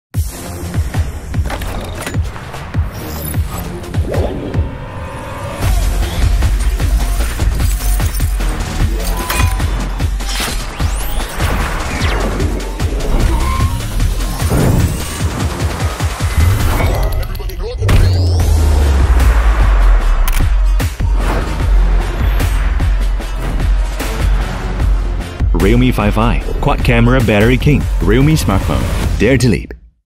realme 5i | The Quad Camera Battery King
Where real craftsmanship meets the best-in-class product experience! Presenting TheQuadCameraBatteryKing realme5i with amazing Sunrise Design, massive 5000mAh Battery, Ultra-Wide Quad Camera and a powerful Snapdragon 665 processor.